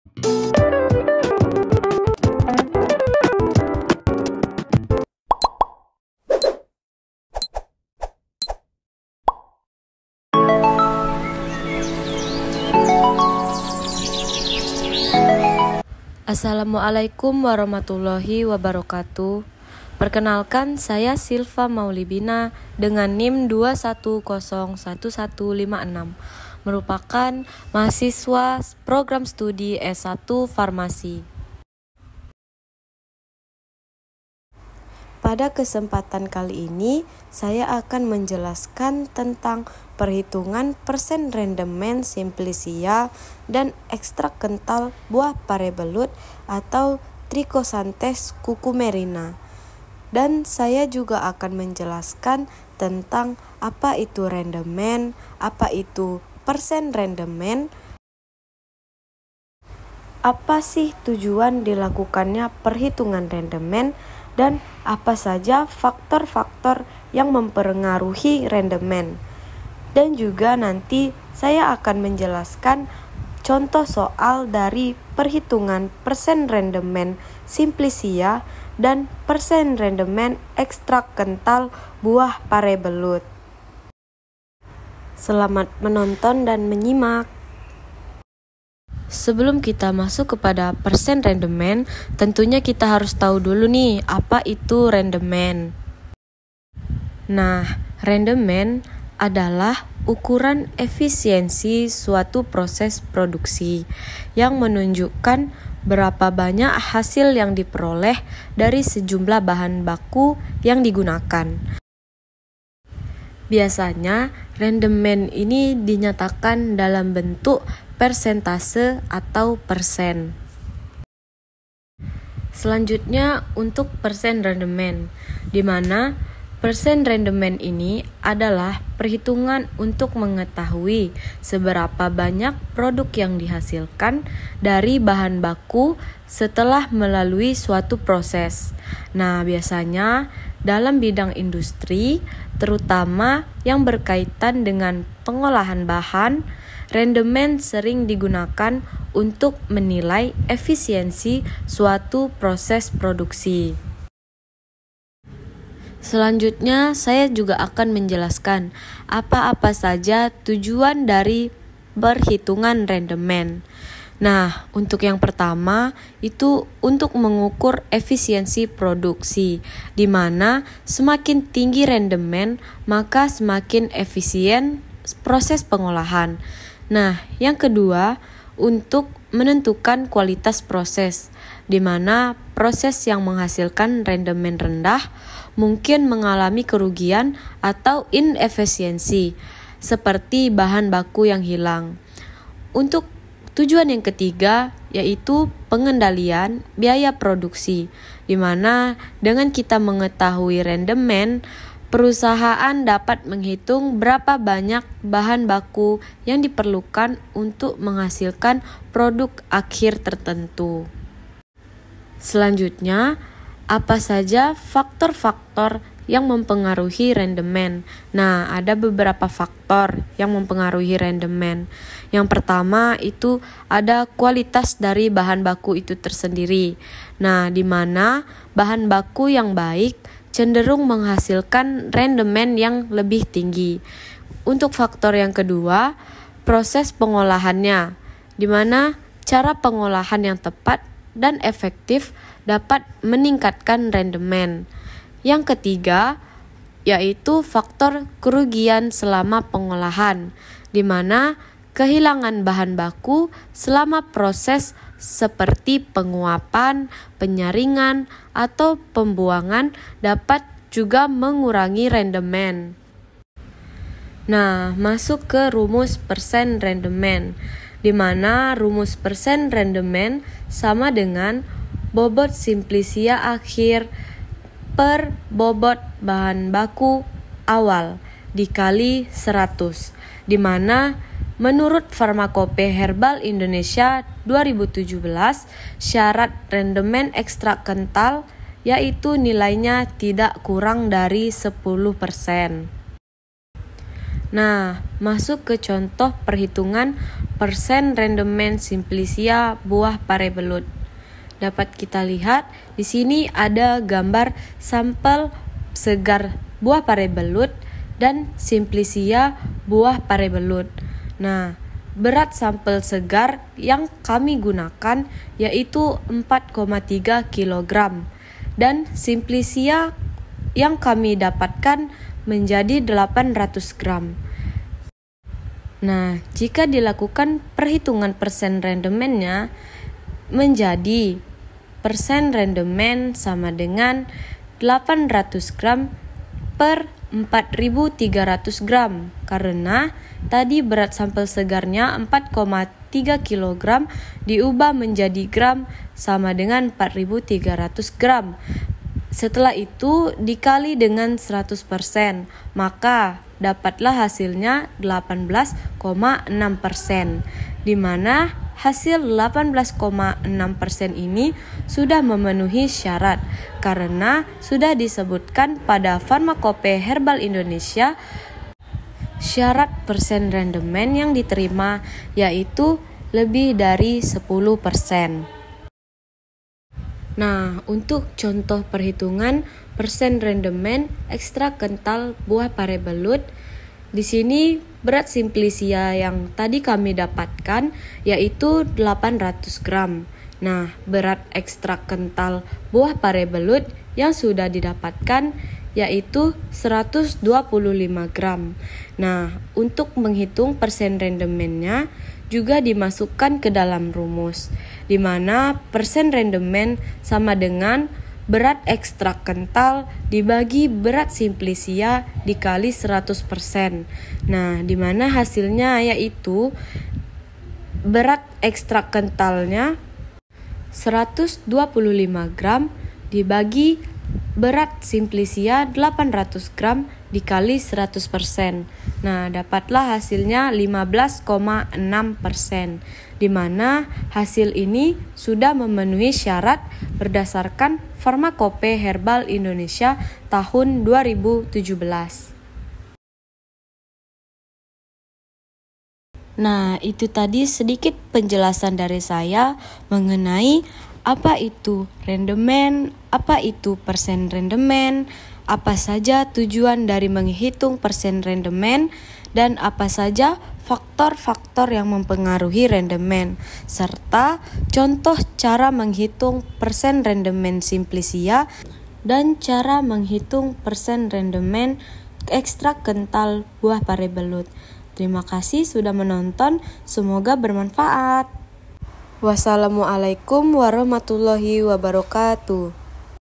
Cara Perhitungan % Rendemen Simplisia dan Ekstrak
Silva Maulibina - 2101156 - Perhitungan % Rendemen Simplisia dan Ekstrak Kental Buah Pare Belut.mov